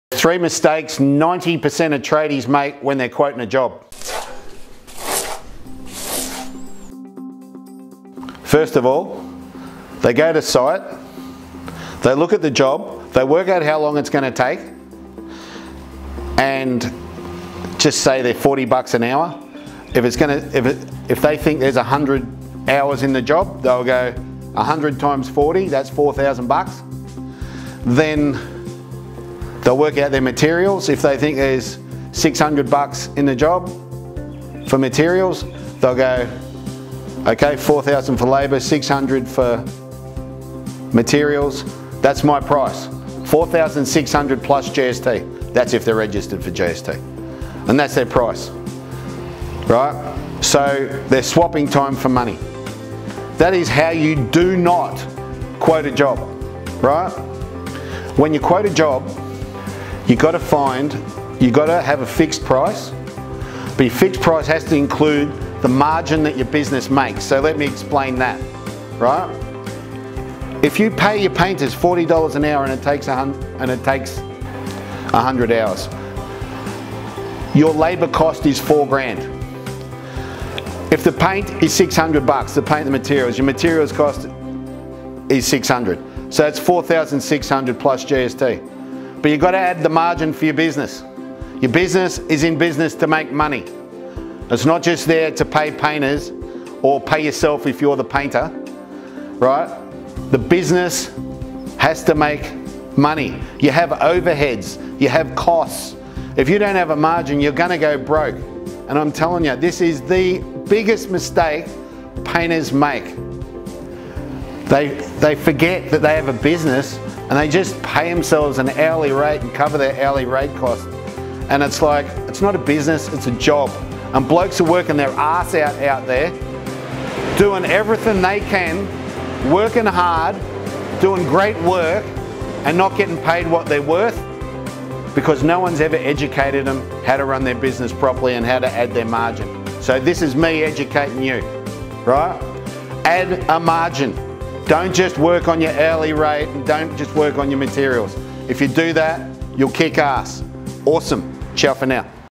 The 3x mistakes 90% of tradies make when quoting a job
Some common mistakes that I see happen a lot in our industry...